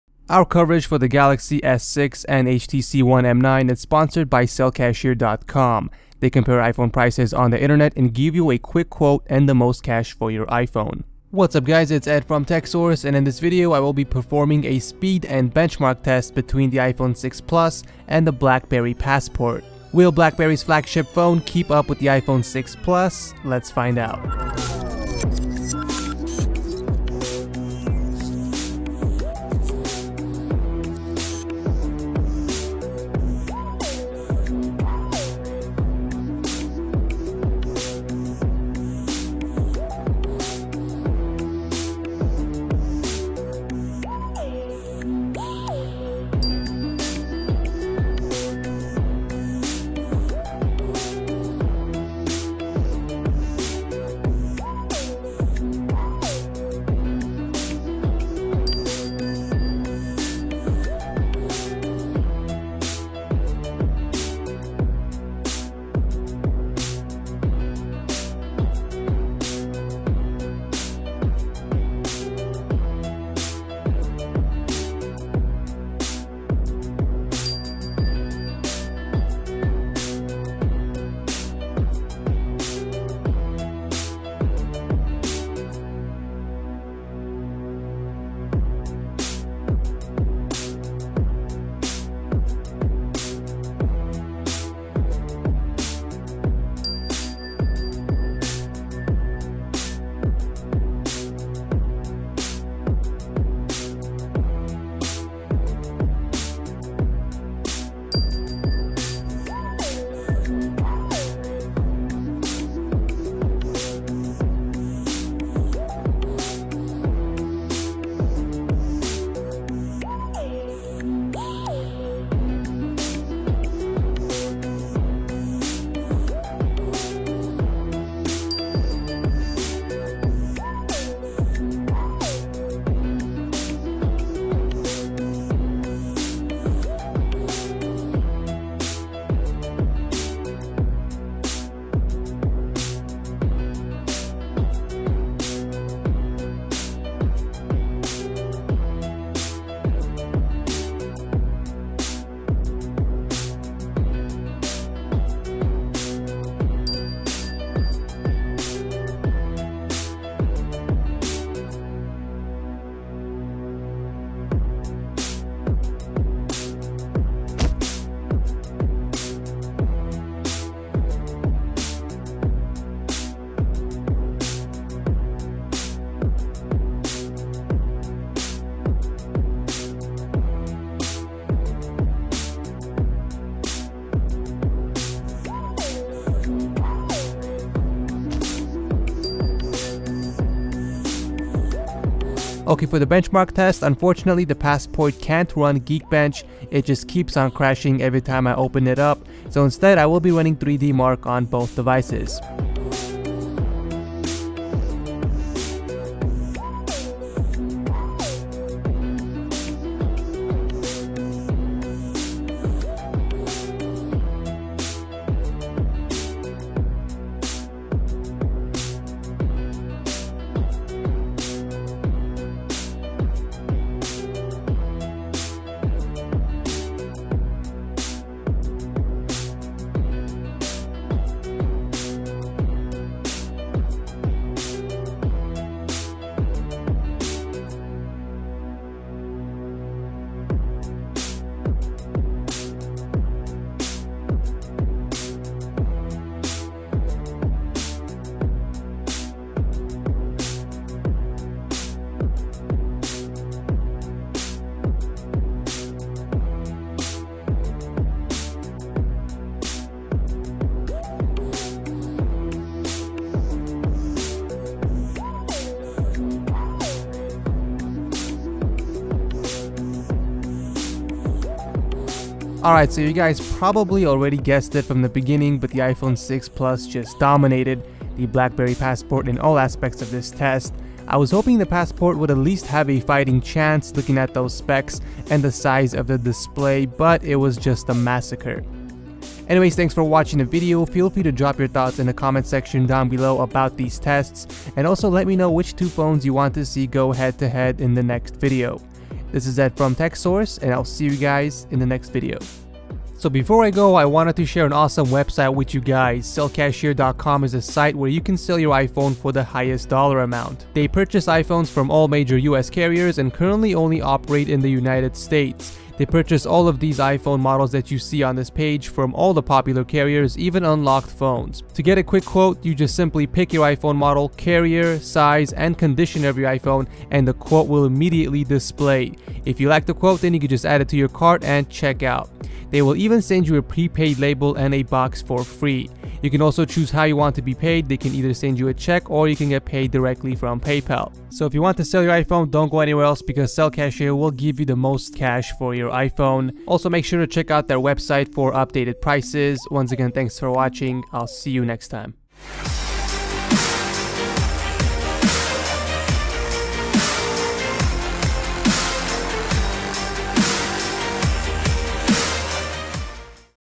iPhone 6 Plus vs Blackberry Passport - Speed test + Benchmarks
Performing a speed test and benchmark test between the iphone 6 plus and the blackberry passport.

My Office:*Coming Soon*